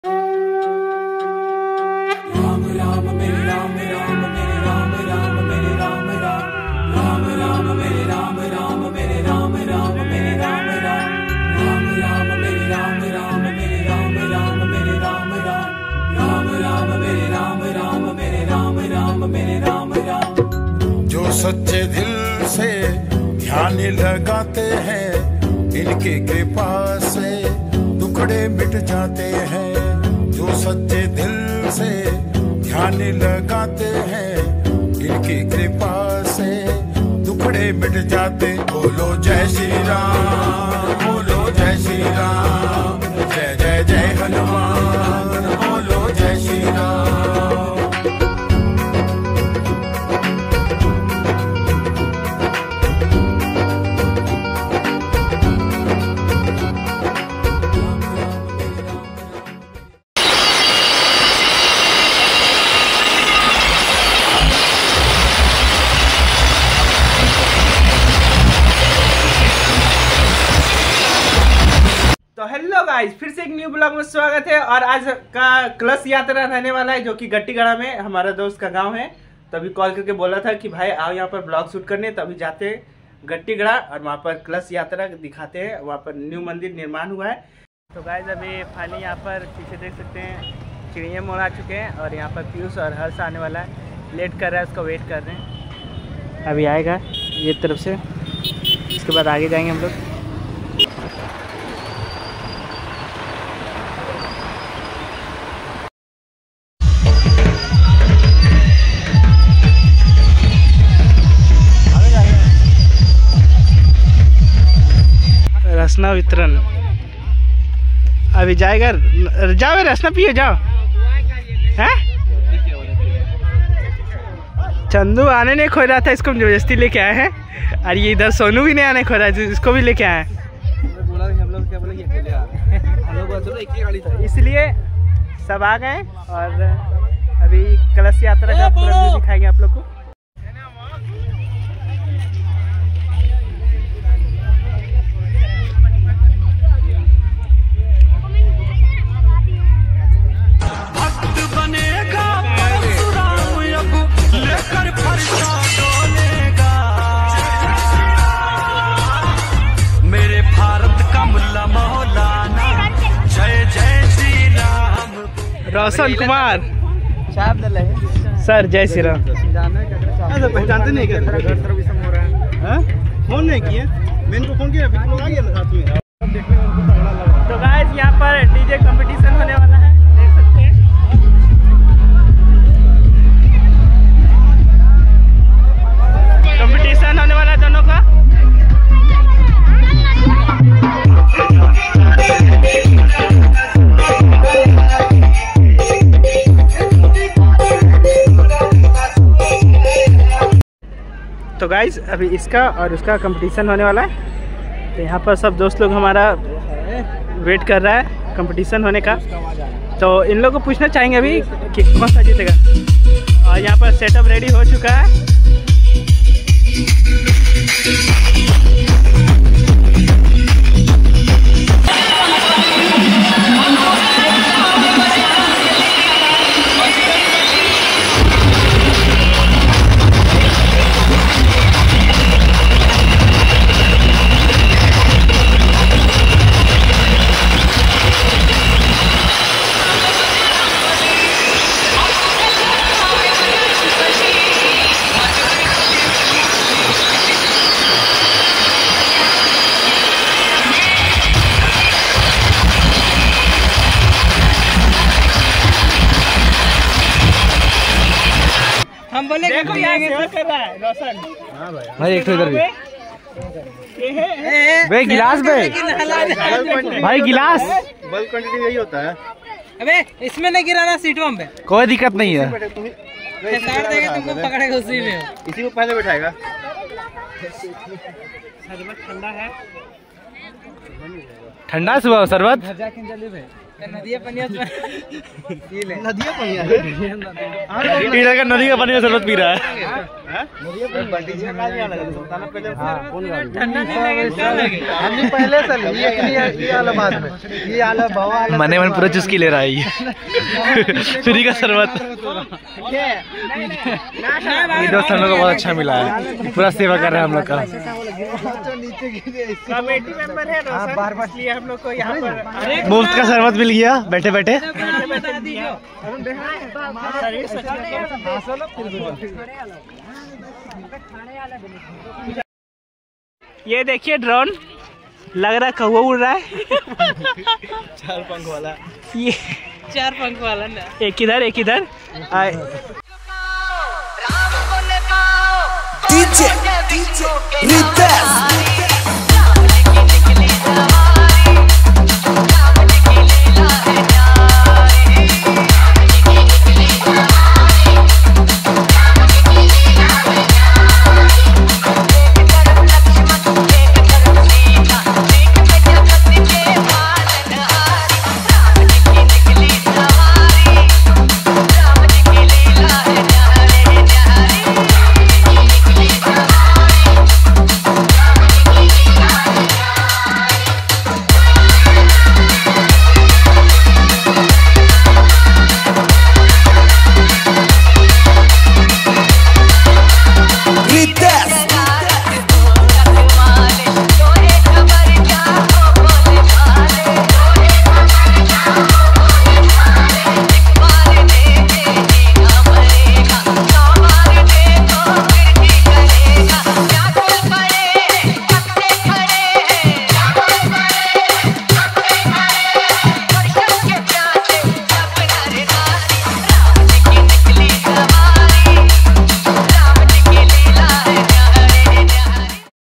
🏺कलश यात्रा 🚩 जय श्री राम 🚩 || Gattigarha Hanuman mandir vlog 🙏
आज गट्टीगढ़ा कलश यात्रा में Dj mani और Dj shivam के साथ हो गया competition 💪🤟

⚜️Thank you so much for the watching my Full video😘

⚜️Aapka dost 💝Shubham💝

⚜️DUMRI WALA VLOGS 🧡🤍💚
         From,
                  Dumri (Giridih) Jharkhand 🇮🇳